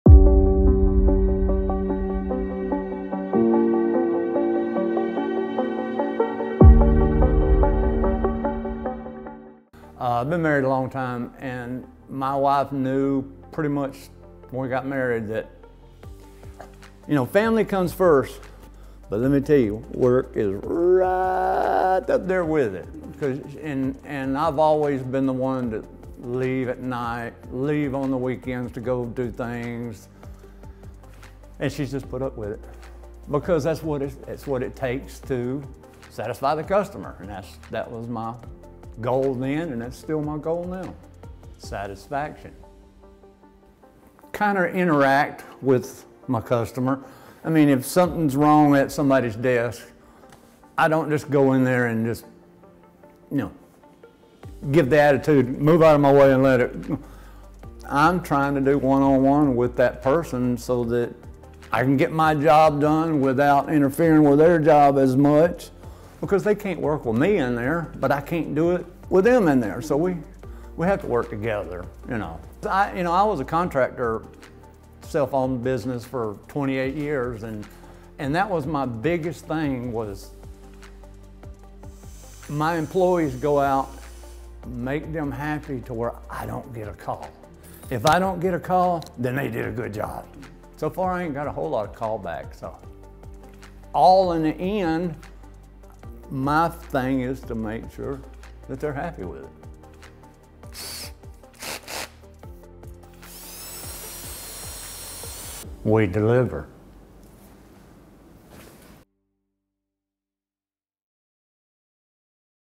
Douglas Delivers with Allen Mashburn
Meet Allen Mashburn, the Douglas County Courthouse’s Chief Engineer and Electrician! Learn how he delivers!